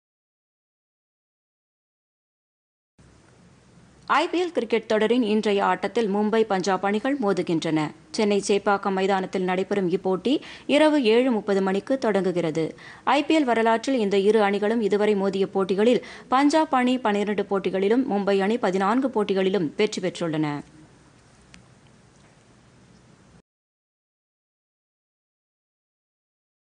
ஐபிஎல் கிரிக்கெட்PodhigaiTamilNews
ஐபிஎல் கிரிக்கெட் :  மும்பை - பஞ்சாப் அணிகள் இன்று பலப்பரீட்சை.

Visit : TamilNews - Doordarshan